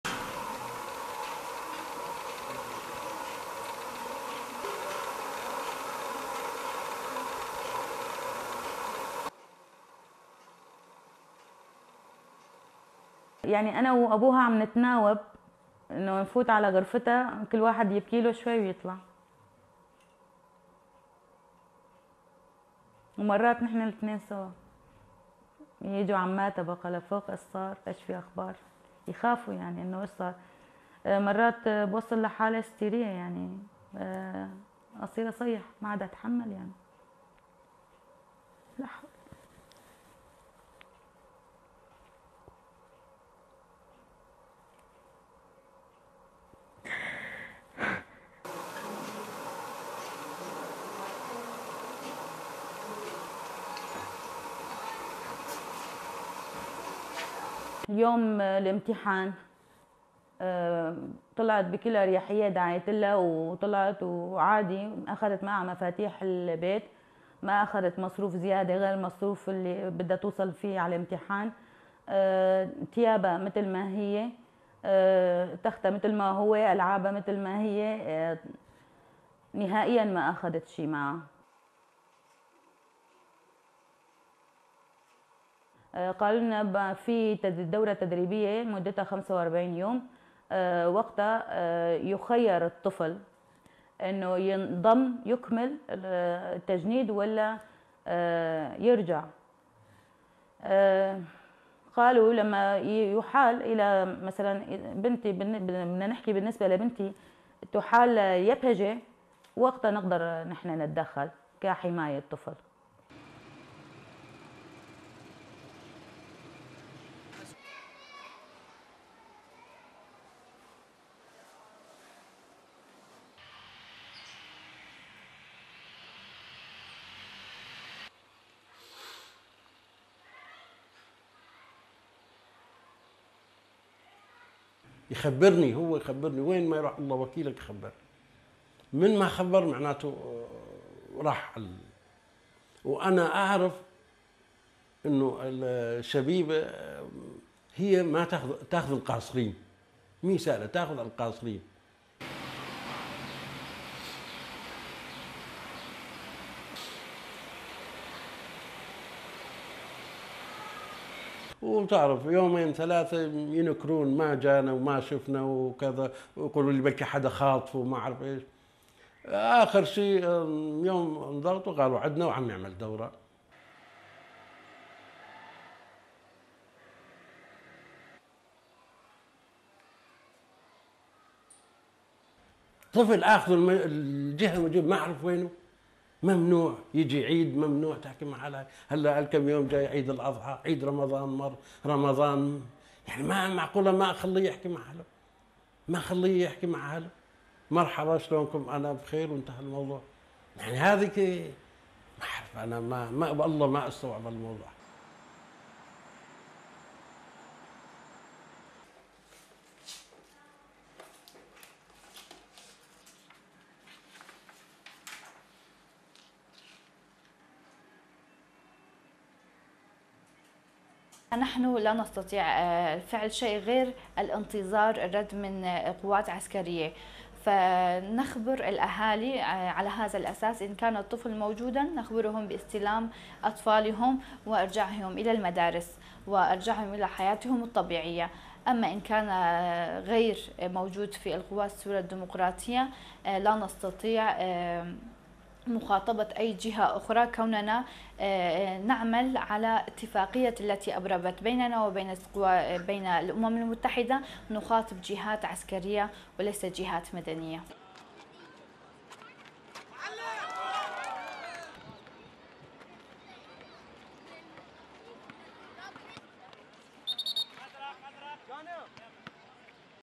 Parents: Syrian Kurdish teens taken by armed group
SYRIA CHILD RECRUITMENT

RESTRICTION SUMMARY:

LENGTH: 4:30

ASSOCIATED PRESS

Qamishli, Syria - 27 May 2023

1. Various of bed and teddy bears in room of 13-year-old Peyal Aqil, who was taken from school and has been missing
2. SOUNDBITE (Arabic) Hamrin Alouji, Peyal's mother:
"Her father and I take turns entering her room. Each of us cries a little, then leaves. Sometimes we both cry together. Relatives come and ask for updates, they are worried. Sometimes, I find myself in a state of hysteria, screaming. I can't handle this, I don't have the strength…(crying)."

3. Peyal's bedroom
4. Peyal's books and belongings on bed
5. SOUNDBITE (Arabic) Hamrin Alouji, Peyal's mother:
"On the day of her exam, she left the house at ease, and I prayed for her. She took the house keys with her, but did not take extra money - only just enough to get to the exam hall. Her clothes, her bed, and her toys were left untouched. She didn't take anything else with her."

6. Peyal's books and belongings
7. SOUNDBITE (Arabic) Hamrin Alouji, Peyal's mother:
"They told us that she's enrolled in a training program that lasts 45 days. After that, the child will be given the option of either continuing with the recruitment process or returning home. As for my daughter, they said, 'if she was assigned to the YPJ (Kurdish Women's Protection Units), then we could step in under child protection."

Qamishli, Syria - 30 May 2023

8. Teenage boy passing by mural about children's rights
9. Walid al-Halabi, father of 15-year-old Omar, who went missing
10. Al-Halabi looking at pictures of his son on mobile
11. Close of al-Halabi wiping his tears
12. SOUNDBITE (Arabic) Walid al-Habib, Omar's father:
"He (my son) always told me whenever he planned to go somewhere. The last time, he didn't, which means he has gone (missing). And I know that the (Revolutionary) Youths are taking minors. It's that party which takes (recruits) minors."

13. Various of al-Halabi looking at phone
14. SOUNDBITE (Arabic) Walid al-Habib, Omar's father:
"You know, for two or three days they denied it, saying 'he hasn't come to us, and we haven't seen him.' They suggested that someone might have kidnapped him and the likes. Finally, one day, under pressure, they admitted, 'he is with us, enrolled in a training program.'"

15. Wide of al-Habib sitting in living room, looking at phone
16. Omar’s schoolbooks in living room
17. SOUNDBITE (Arabic) Walid al-Habib, Omar's father:
"An unknown party has taken my child. I don't know his whereabouts. He's not allowed to visit home during Eid, and I'm not permitted to speak with him. The Eid al-Adha (holiday) is around the corner. And (the fasting month of) Ramadan and Eid (al-Fitr holiday) have already passed. It's absurd that they won't let him speak to his family, not even to say a simple 'hi, I am fine.' I just can't comprehend this."

18. Boys walking past mural about children's rights

Qamishli, Syria - 29 May 2023

19. Various of Nodem Shero of the Child Protection Office in the Kurdish administration in northeast Syria, looking through files
20. Files and binder with text reading (Arabic): "Incoming complaints - 2023"
21. SOUNDBITE (Arabic) Nodem Shero, Child Protection Office at the Kurdish administration in northeast Syria:

22. Various of children playing basketball
STORYLINE:

The Revolutionary Youth is a group linked to the Kurdistan Worker’s Party, or PKK, a Kurdish separatist movement banned in Turkey.

Aqil's friends waited for her outside, but she never emerged.